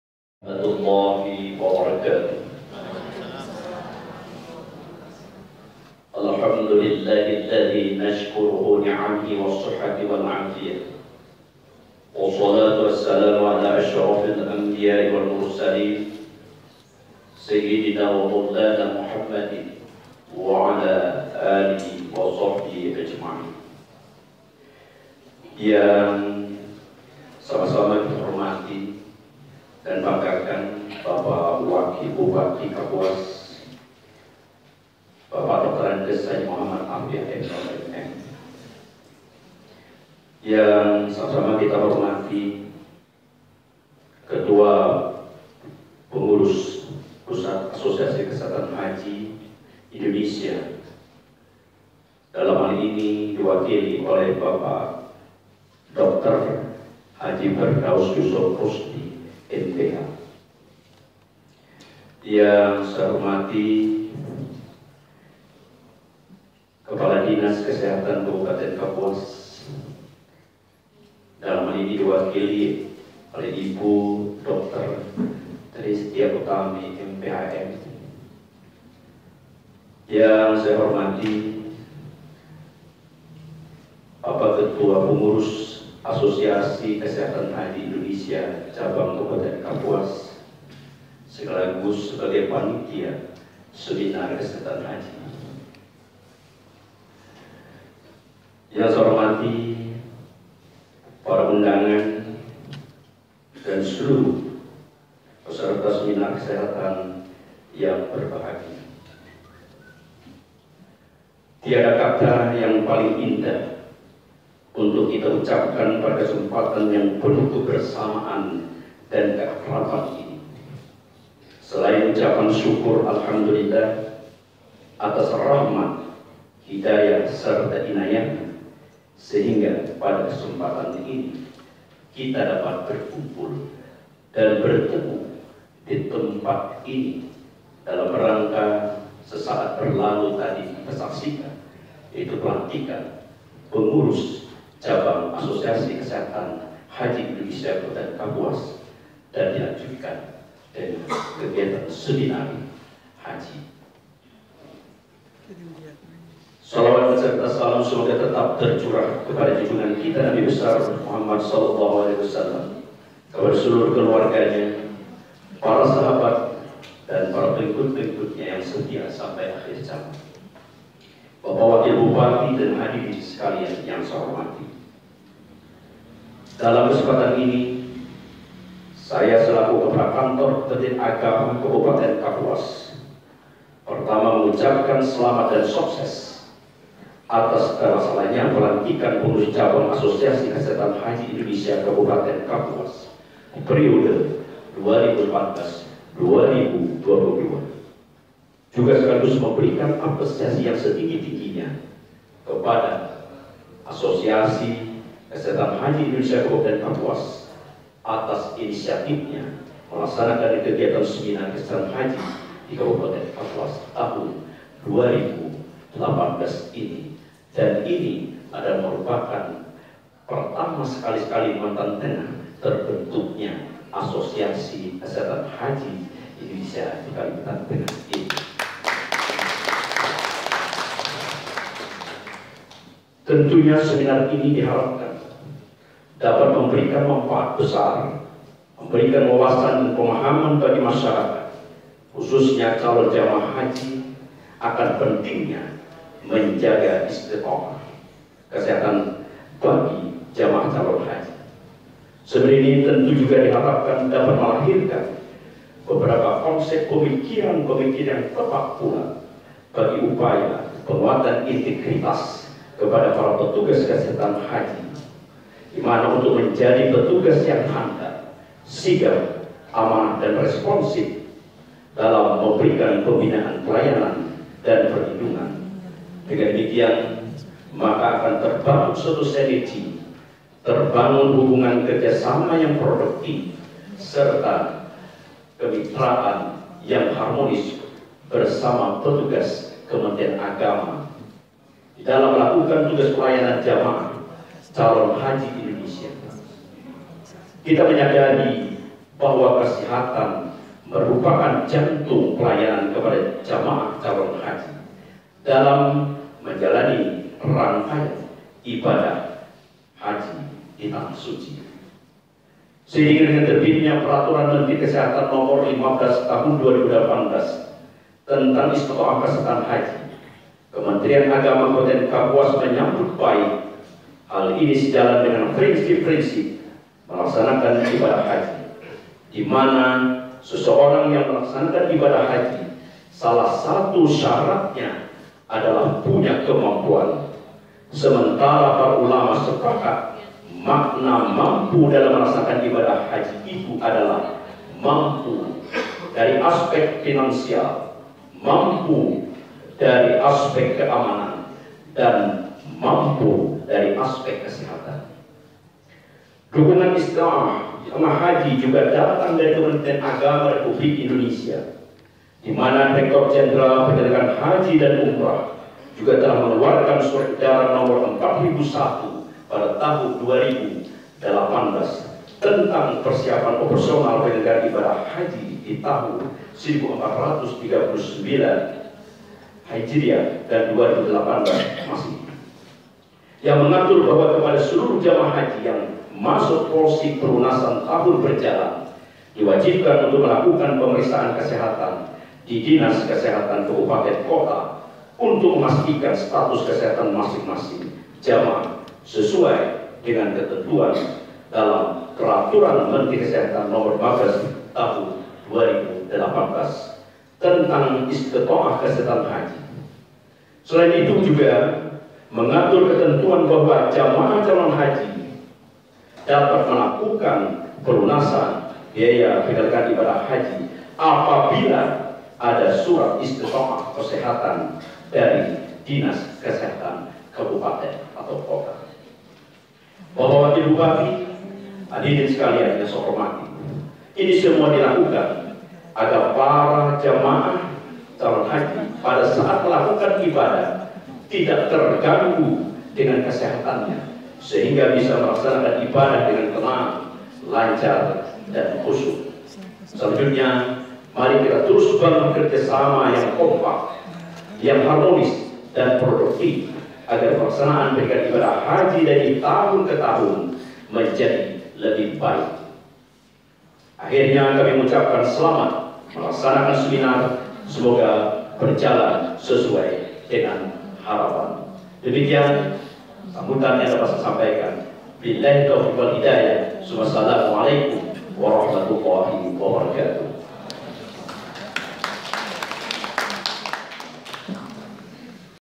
Sambutan Kepala Kantor Kementerian Agama pada Seminar Kesehatan Haji 2018
INFORMASI KAPUAS - Disampaikan oleh Bapak Basuni pada hari Ahad, 18 November 2018 di aula Bappeda Kabupaten Kapuas, Kalimantan Tengah.